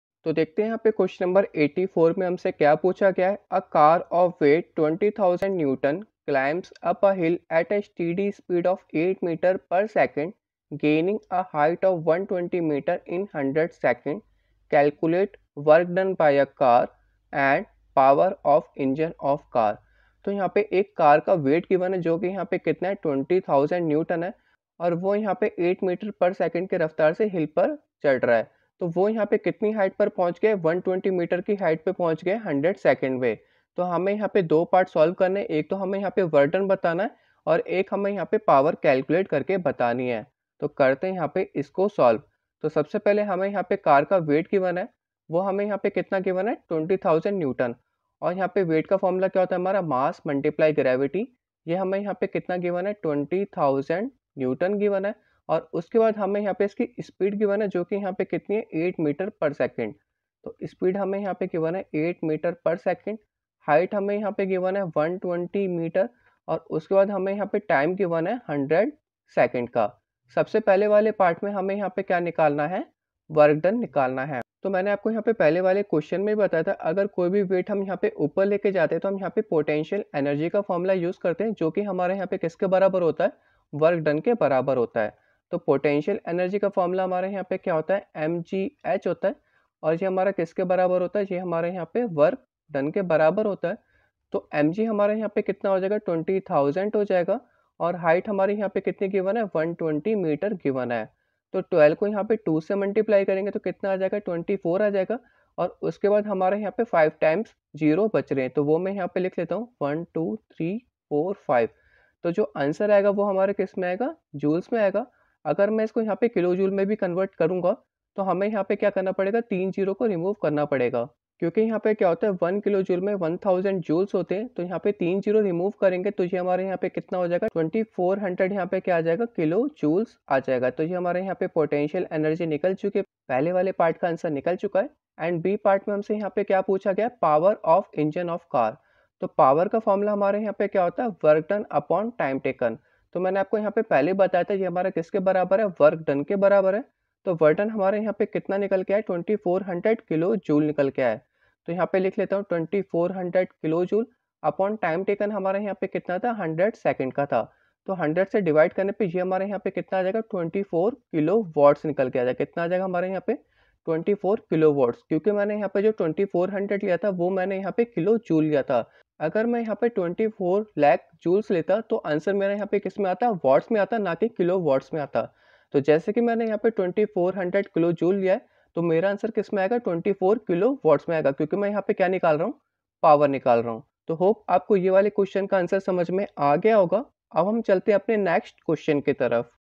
A car of weight 20000 N climbs up a hill at a steady speed of 8 m/s gaining a height of 120m in 100s
Q.84 A car of weight 20000 N climbs up a hill at a steady speed of 8 m/s, gaining a height of 120 m in 100 s. Calculate : (a) work done by the car. (b) power of engine of car.